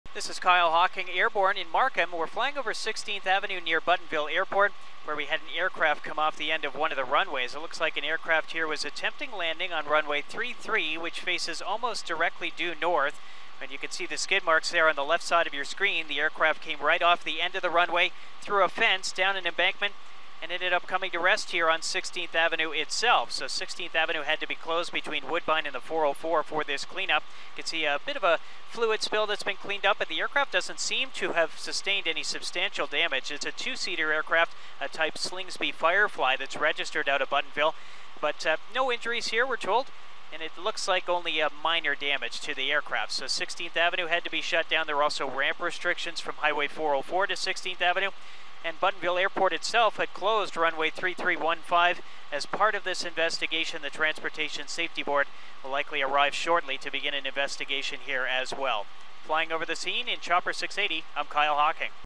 Small plane crashes off runway at Buttonville Airport
Vehicles had to avoid a small plane that ended up on the road adjacent to Buttonville Airport on Monday afternoon. The plane was seen on 16th Avenue, just east of Highway 404, near the airport in Markham.